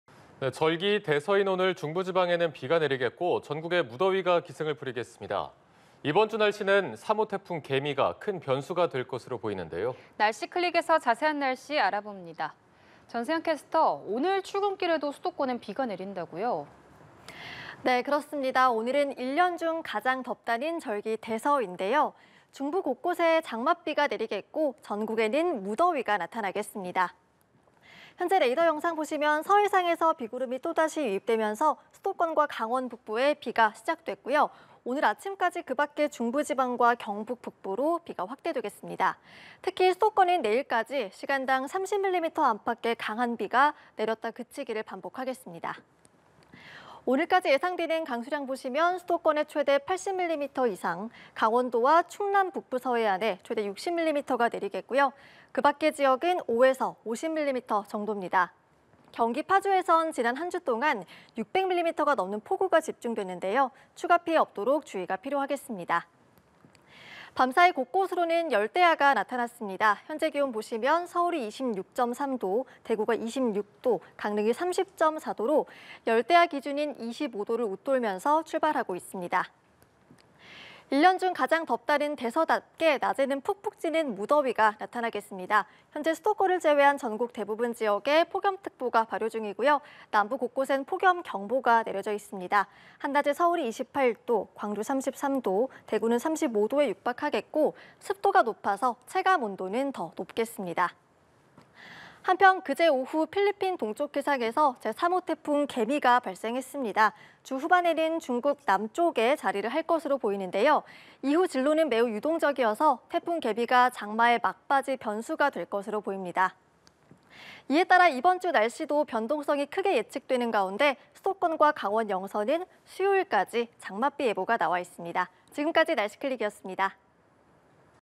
[날씨] 절기 '대서'…중부 장맛비, 전국 무더위 기승 / 연합뉴스TV (YonhapnewsTV)
[날씨] 절기 '대서'…중부 장맛비, 전국 무더위 기승

[앵커]
절기 '대서'인 오늘 중부지방엔 비가 내리겠고, 전국에 무더위가 기승을 부리겠습니다.

이번 주 날씨는 3호 태풍 '개미'가 큰 변수가 될 것으로 보이는데요.

날씨클릭에서 자세한 날씨 알아봅니다.

전세영 캐스터, 오늘 출근길에도 수도권에는 비가 내린다고요?

[캐스터]

오늘은 1년 중 가장 덥다는 절기, '대서'인데요.

중부 곳곳에 장맛비가 내리겠고, 전국에는 무더위가 나타나겠습니다.

현재 레이더영상 보시면, 서해상에서 비구름이 유입되면서 수도권과 강원 내륙에 비가 시작됐고요.

오늘 아침까지 그 밖의 중부지방과 경북북부로 점차 확대되겠습니다.

특히 수도권은 내일까지, 시간당 30mm 안팎의 강한 비가 내렸다 그치기를 반복하겠습니다.

오늘까지 예상되는 강수량은, 수도권에 최대 80mm 이상, 강원과 충남북부서해안에 60mm 이상이 내리겠고, 그 밖의 지역은 5에서 50mm 정돕니다.

경기 파주에서는 지난 한 주 동안, 600mm가 넘는 폭우가 집중됐습니다.

추가 피해 없도록 주의가 필요하겠습니다.

밤사이, 곳곳으로는 '열대야'가 나타났습니다.

현재 기온 보시면, 서울이 26.3도, 대구 26도, 강릉 30.4도로 열대야 기준인 25도를 웃돌며 출발하고 있습니다.

1년 중 가장 덥다는 '대서'답게, 낮에는 푹푹 찌는 무더위가 나타나겠습니다.

현재 수도권을 제외한 전국으로 폭염특보가 발효 중이고요.

남부 곳곳엔 '폭염경보'가 발효 중입니다.

한낮에 서울이 28도, 광주 33도, 대구는 35도에 육박하겠습니다.

습도가 높아 체감온도는 더 높겠습니다.

한편, 그제 오후 필리핀 동쪽 해상에서 제3호 태풍 '개미'가 발생했습니다.

주 후반에는 중국 상하이 남쪽에 상륙할 것으로 보이는데요.

이후 진로는 매우 유동적이어서 태풍 '개미'가 장마의 막바지 변수가 될 것으로 보입니다.

이에 따라 이번 주 날씨도 변동성이 크게 예측되는 가운데, 수도권과 강원 영서는 수요일까지 장맛비 예보가 나와 있습니다.

지금까지 날씨클릭이었습니다.

(전세영 기상캐스터)